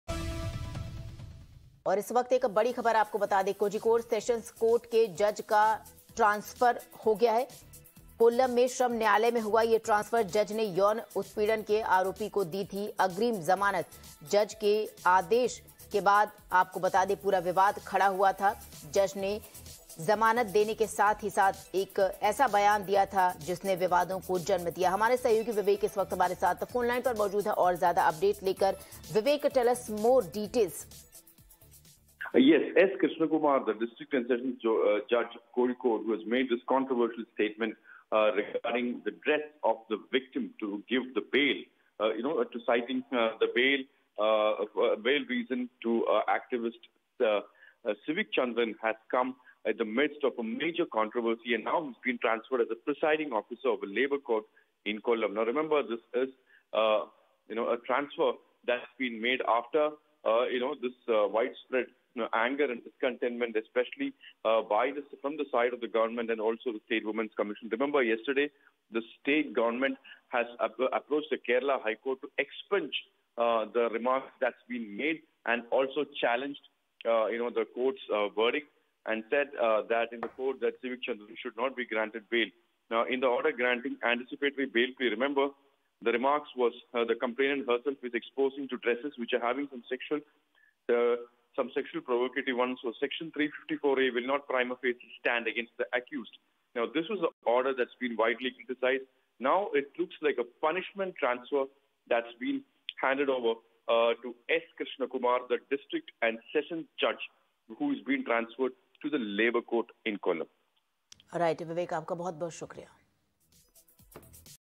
Kozhikode District Judge Known For Granting Bail To Rape Accused Transferred | Latest News
S Krishnakumar, a notorious district and sessions judge from Kozhikode who had granted bail in two sexual harassment cases to an accused, has been promoted to preside over the labor court in Kollam. Muralee Krishna S, the District and Sessions Judge for Manjeri, will take over as the District and Sessions Judge for Kozhikode, under the Kerala High Court's transfer ruling. Two other judges have also been transferred as part of the high court's ordinary administrative business in the judiciary transfer and posting orders of District and Sessions judges that were issued late on Tuesday. Tune in for more such updates and news.